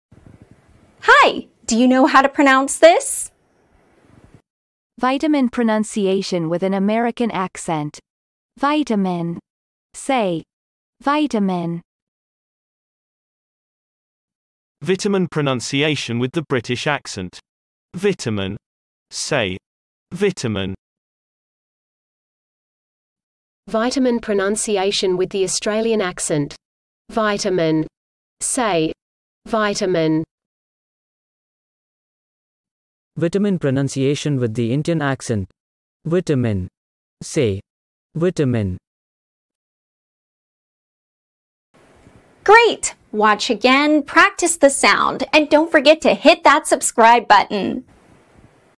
⭐️ 8th 👉Vitamin👈 How To Pronounce Vitamin 🇺🇸 🇬🇧 🇦🇺 🇮🇳 accents
🇺🇸 vs. 🇬🇧 vs. 🇦🇺 vs. 🇮🇳 - English Accent Battle 💥
Listen and learn the perfect English pronunciation of the word `VITAMIN` to take your accent to the next level.

- how to pronounce the word 'VITAMIN' CORRECTLY
- pronunciation of `VITAMIN` in English with an American accent
- pronunciation of `VITAMIN` in English with a British accent
- pronunciation of `VITAMIN` in English with an Australian accent
- pronunciation of `VITAMIN` in English with an Indian accent

Video chapters:
00:00 - Video Intro
00:04 - VITAMIN pronunciation in US (American accent)
00:14 - VITAMIN pronunciation in UK (British accent)
00:23 - VITAMIN pronunciation in AU (Austraian accent)
00:32 - VITAMIN pronunciation in IN (Indian accent)
00:41 - Video End

⌚ SCHEDULE:  Daily New Video

channel.